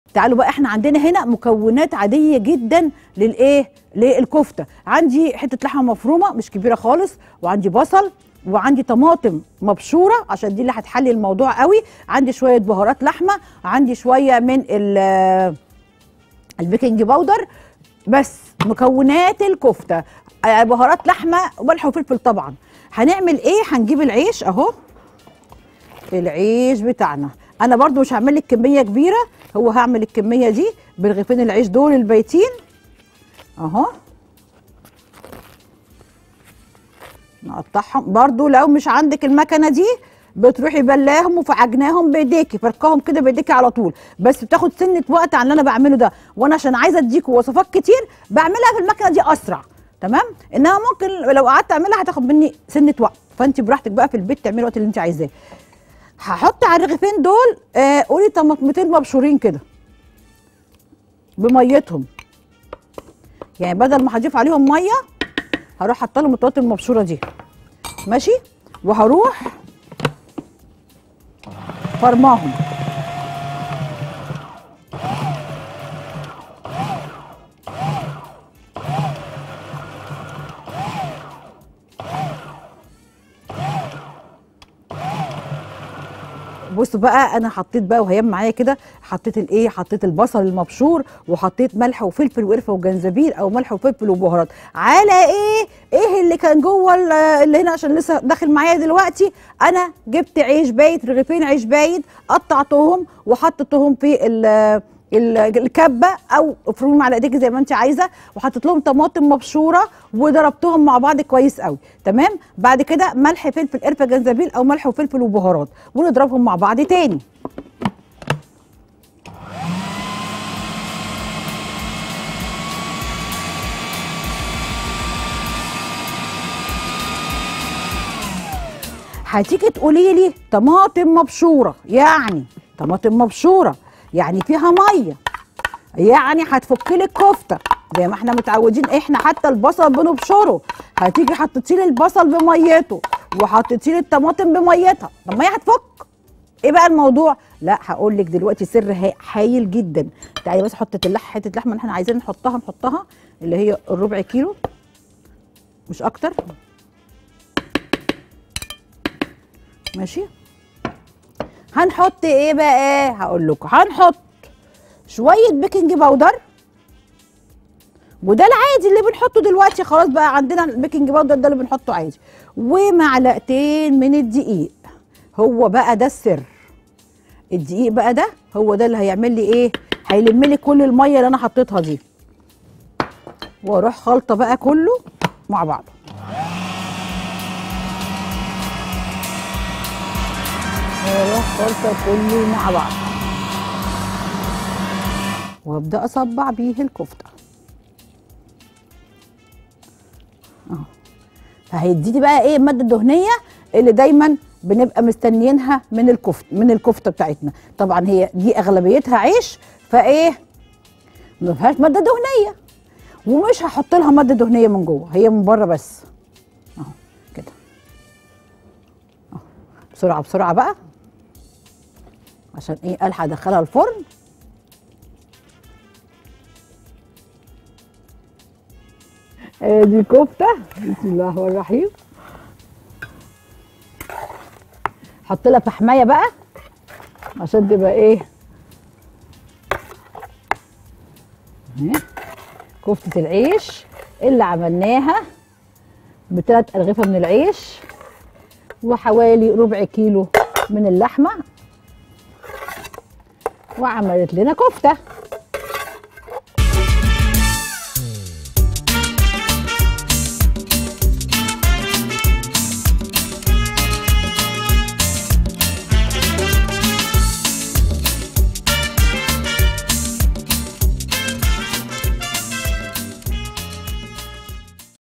كفتة | نجلاء الشرشابي
طريقة عمل كفتة  مع نجلاء الشرشابي على قد الإيد